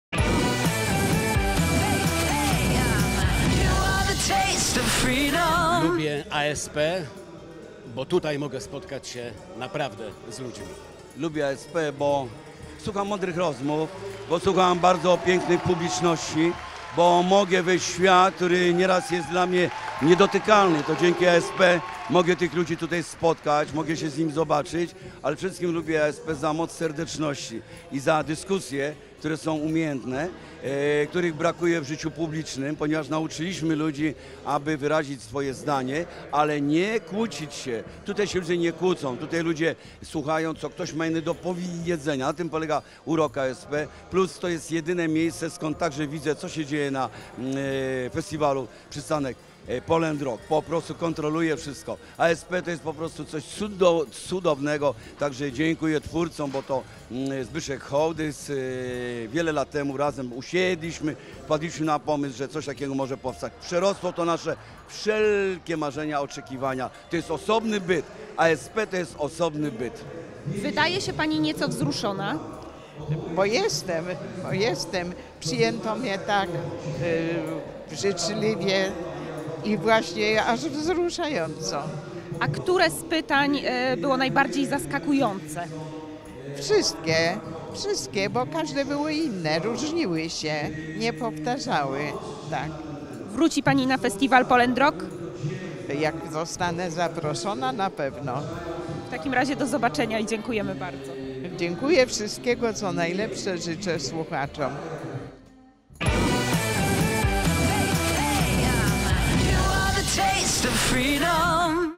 Jurek Owsiak, Arkadiusz Jakubik, Elżbieta Dzikowska - Za co lubimy ASP?
ASP to spotkania z ciekawymi ludźmi, możliwość porozmawiania, zadania pytania, wymiany poglądów w przyjaznej atmosferze. A za co Ty lubisz ASP? 😊